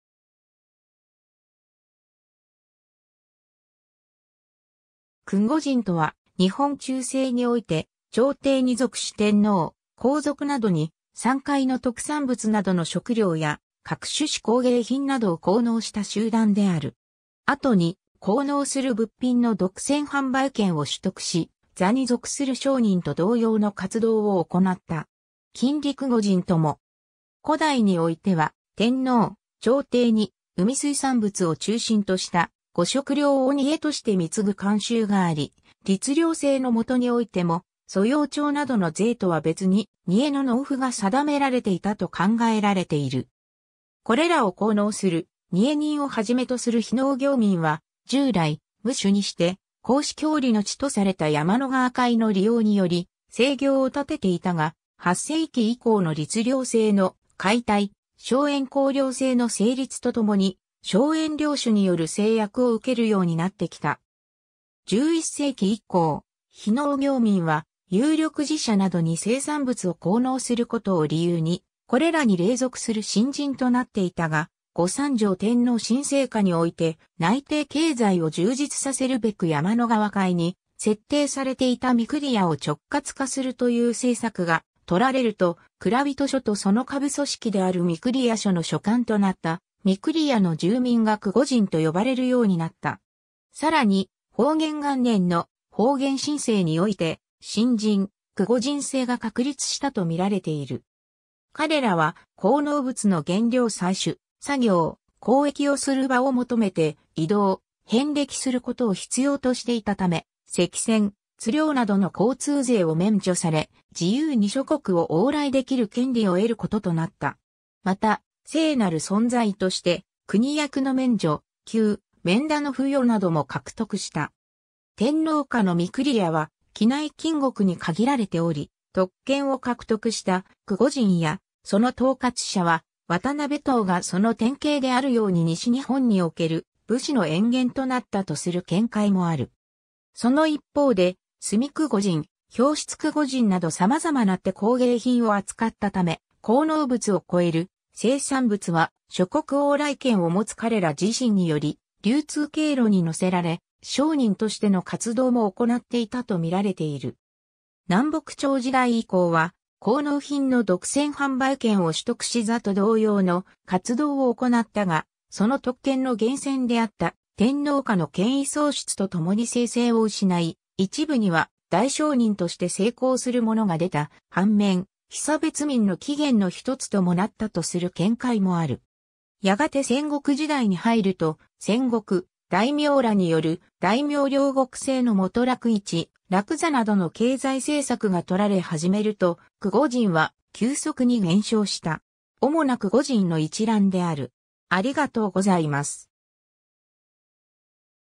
供御人
供御人（くごにん、くごんちゅ）とは、日本中世において、朝廷に属し天皇・皇族などに山海の特産物などの食料や各種手工芸品などを貢納した集団である。
後に、貢納する物品の独占販売権を取得し、座に属する商人と同様の活動を行った。
禁裏供御人とも。
 古代においては、天皇・朝廷に海水産物を中心とした御食料（穀類以外の副食物）を贄（にえ）として貢ぐ慣習があり、律令制のもとにおいても租庸調などの税とは別に、贄の納付が定められていたと考えられている。
 これらを貢納する贄人を初めとする非農業民は、従来「無主」にして「公私共利」の地とされた山野河海の利用により生業をたてていたが、8世紀以降の律令制の解体、荘園公領制の成立とともに、荘園領主による制約を受けるようになってきた。
11世紀以降、非農業民は有力寺社などに生産物を貢納することを理由に、これらに隷属する神人となっていたが、後三条天皇親政下において、内廷経済を充実させるべく山野河海に設定されていた御厨を直轄化するという政策がとられると、蔵人所とその下部組織である御厨所の所管となった御厨の住民が供御人と呼ばれるようになった。
更に、保元元年（1156年）の「保元新制」において神人・供御人制が確立したと見られている。
 彼らは、貢納物の原料採取・作業・交易をする場を求めて移動・遍歴することを必要としていたため、関銭・津料などの交通税を免除され、自由に諸国を往来できる権利を得ることとなった。
また、聖なる存在として国役の免除、給免田の付与なども獲得した。
 天皇家の御厨は畿内近国に限られており、特権を獲得した供御人やその統括者は渡辺党がその典型であるように西日本における武士の淵源となったとする見解もある。
その一方で炭供御人、氷室供御人など様々な手工芸品を扱ったため、貢納物を超える生産物は諸国往来権を持つ彼ら自身により流通経路に載せられ、商人としての活動も行っていたと見られている。
 南北朝時代以降は、貢納品の独占販売権を取得し座と同様の活動を行ったが、その特権の源泉であった天皇家の権威喪失とともに聖性を失い、一部には大商人として成功する者が出た反面、被差別民の起源のひとつともなったとする見解もある。
 やがて戦国時代に入ると、戦国大名らによる大名領国制のもと楽市・楽座などの経済政策が執られ始めると、供御人は急速に減少した。
 おもな供御人の一覧である。